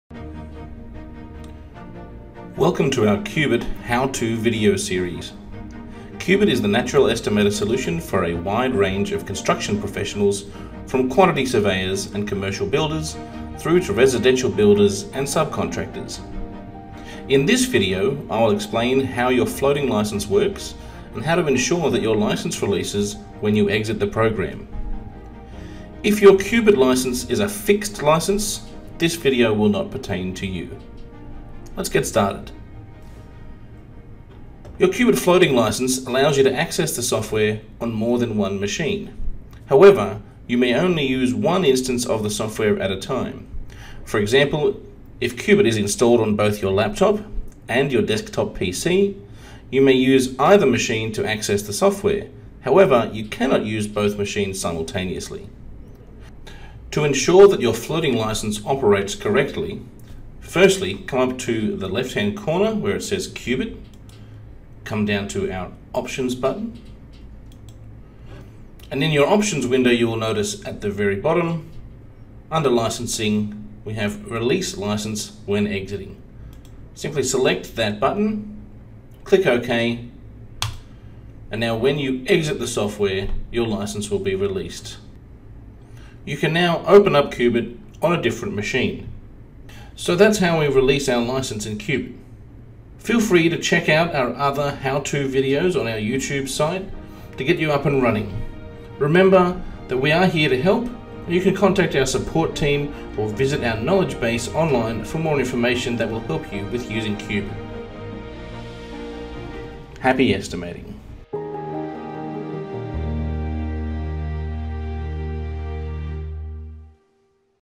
How to release your floating licence in Cubit
Learn how to release your licence in Cubit with this short tutorial. Performing your estimating and takeoff in Cubit it easy. When it comes to using your licence, you’ll need to know how to release it from one computer to use on another. This tutorial runs you through how your floating licence works, and how to ensure it releases when you exit Cubit.

Developed by Buildsoft, Cubit is industry leading estimating software designed to help you improve the efficiency, speed, and accuracy of your construction estimating.